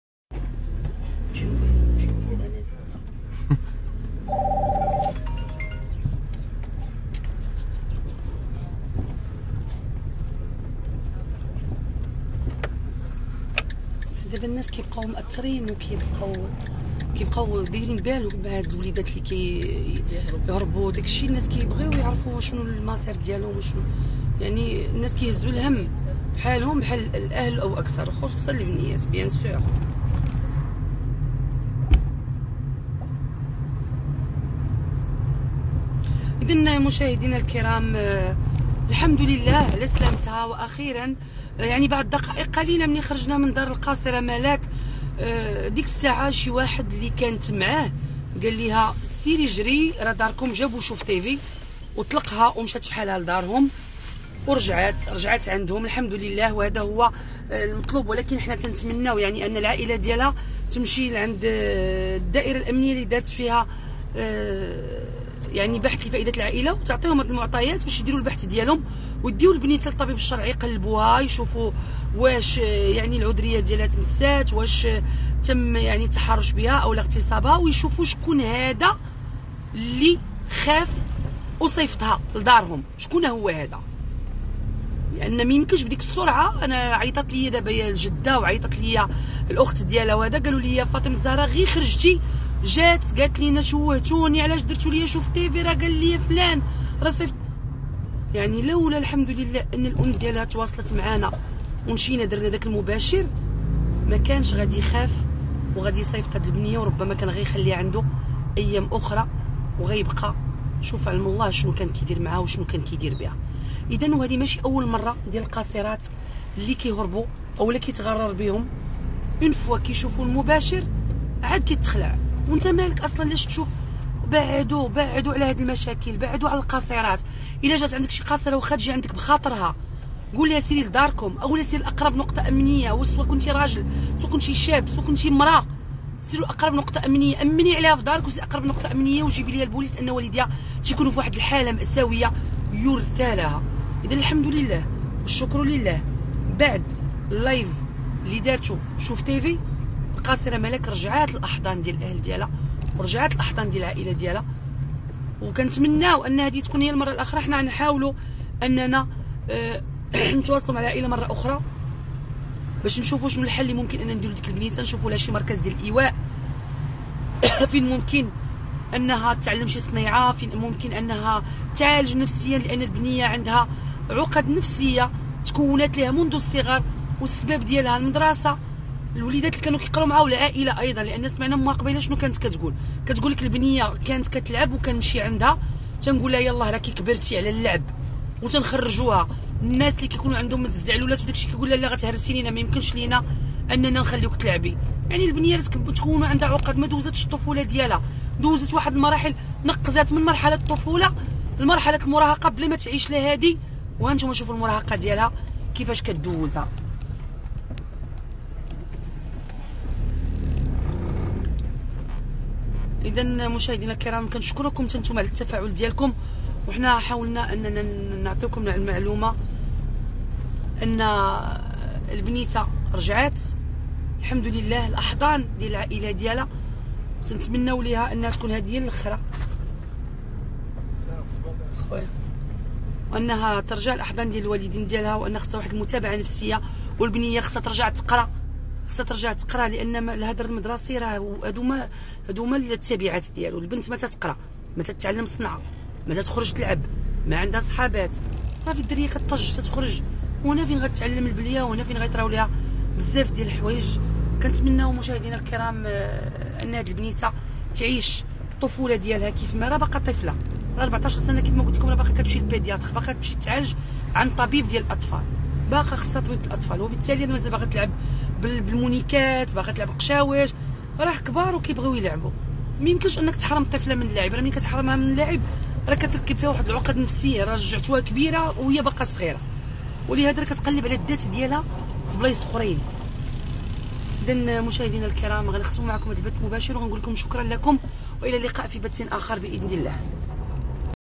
القاصر ملاك لي خرجات تتسخر واختفت شافت لايف قناة الشعب ورجعات لدارهم ولي كان شادها خاف و طلقها
عاجل ومباشرة من الدار البيضاء:دقائق فقط على لايف شوف تيفي..القاصر ملاك لي خرجات تتسخر واختفت شافت لايف قناة الشعب ورجعات لدارهم ولي كان شادها خاف و طلقها